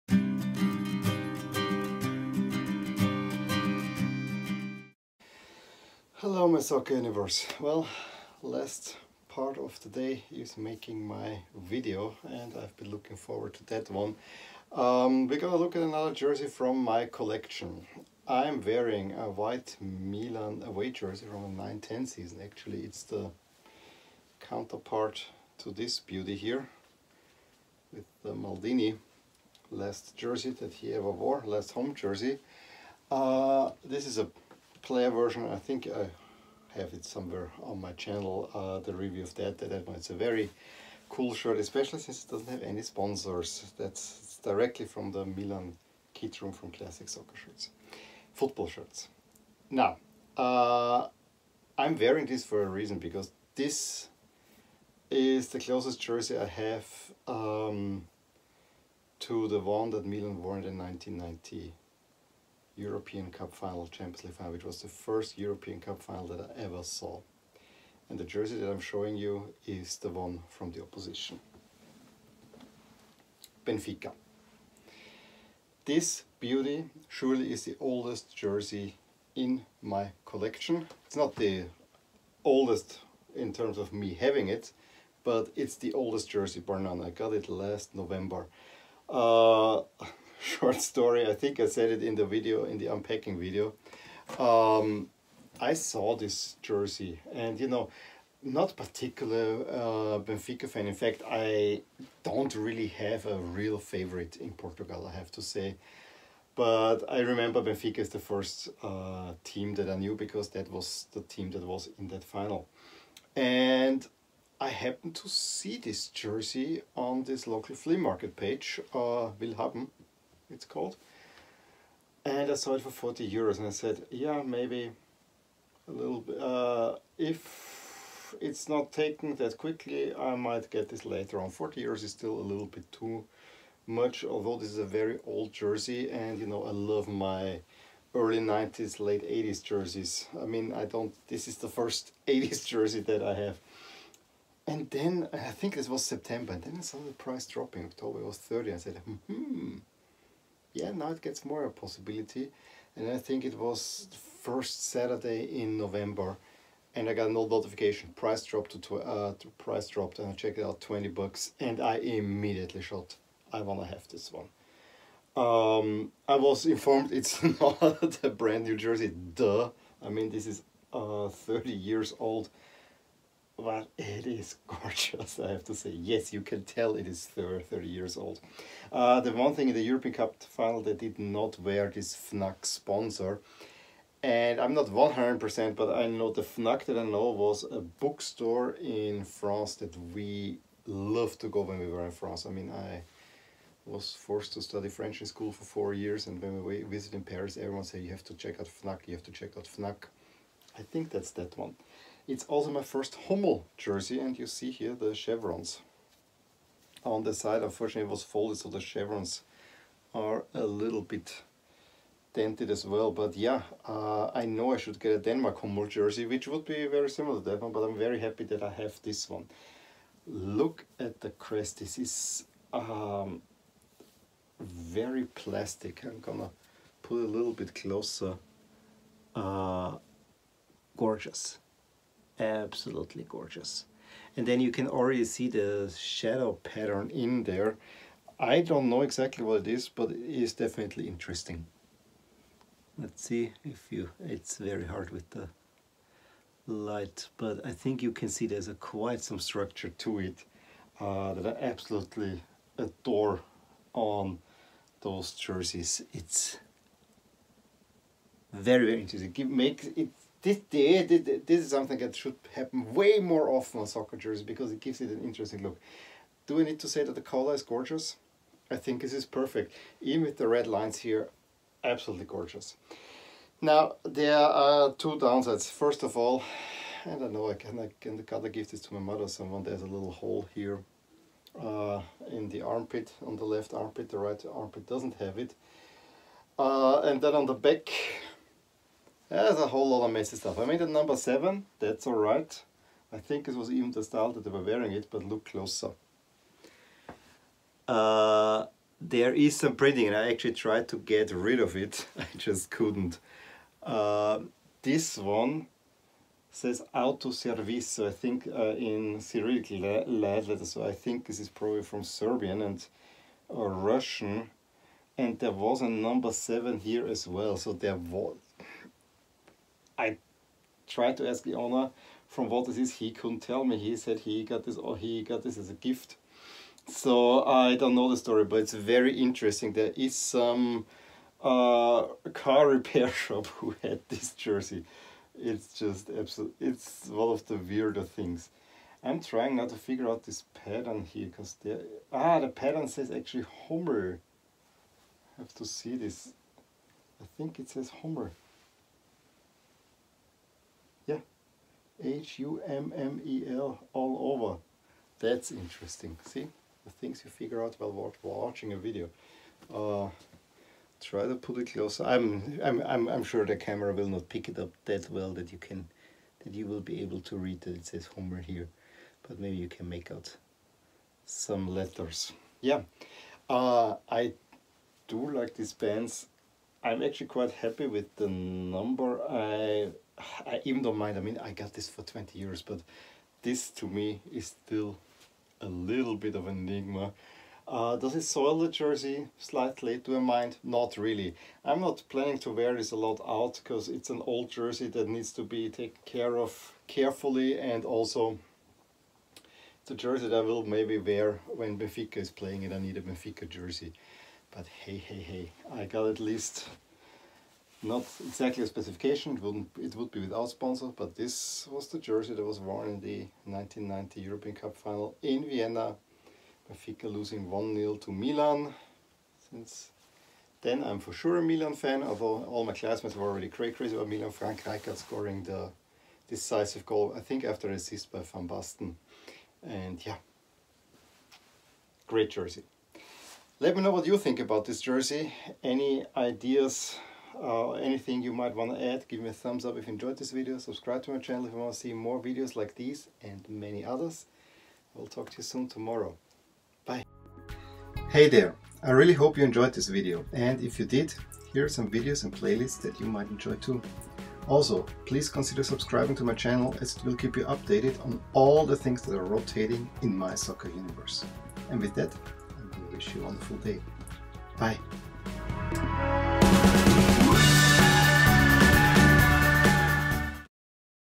SL Benfica (Home 1990-91)
This has to be the oldest jersey in my collection. Note, oldest not the first since I just got it this past November as a steal. And yes, it has some strange markings on the back. But other than that, this is an awesome jersey!